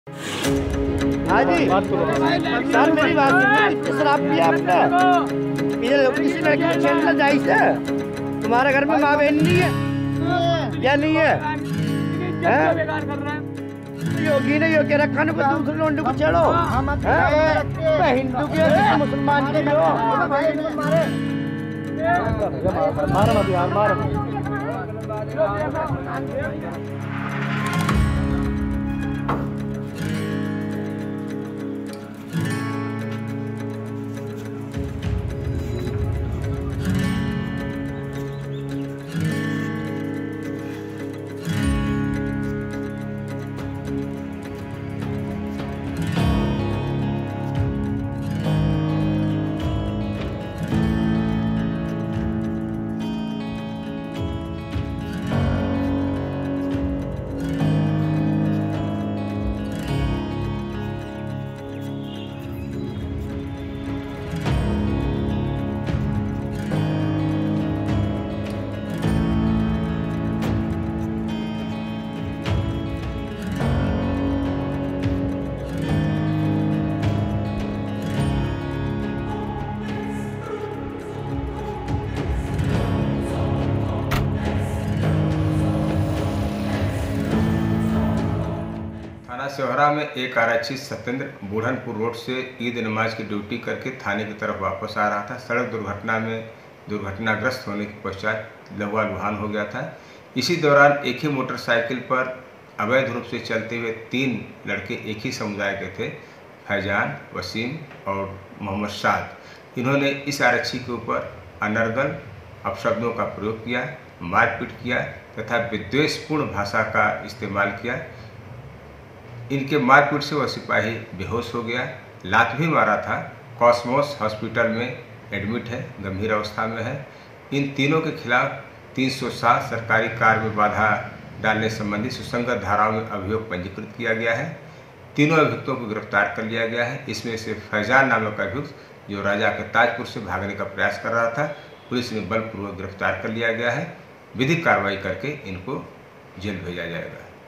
बिजनौर: पुलिस पिटाई की सोशल मीडिया पर वीडियो वायरल,मुकदमा दर्ज
बिजनौर के थाना स्योहारा क्षेत्र में पुलिसकर्मी की पिटाई का वीडियो सोशल मीडिया पर वायरल,वीडियो में ग्रामीण द्वारा पुलिसकर्मी पर शराब के नशे में लड़कियों से छेड़छाड का आरोप लगाते हुए तीन युवकों पिटाई की।पुलिस ने तीनों अभियुक्तों को गिरफ्तार कर लिया।